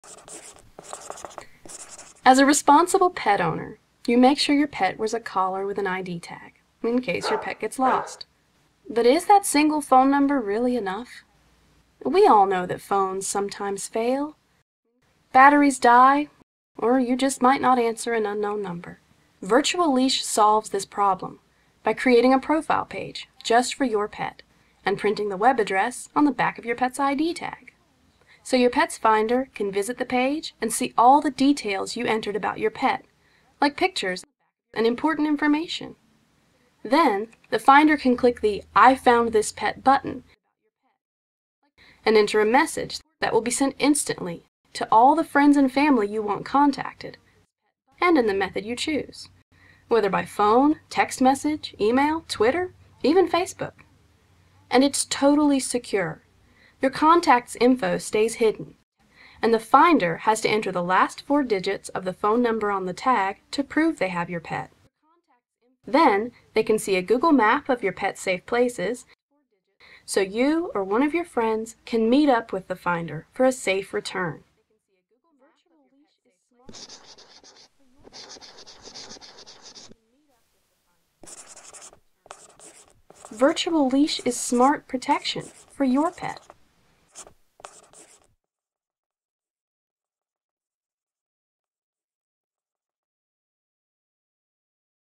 "Virtual Leash in the style of RSA Animate" by Dog Tag Art
Virtual Leash allows you to maintain a unique website (a.k.a. online profile) for your pet with emergency contacts that can be instantly notified. The system can send you, and your emergency contacts, messages through email, social media, text message and voicemail when your pet is found. Virtual Leash also uses Google Maps to show potential finders a safe place for your pet. You can change this address easily and at anytime.

We love the style of RSA animate, so we used some of their creative ideas to build this animation.

to sign up and get yourself a cool ID tag!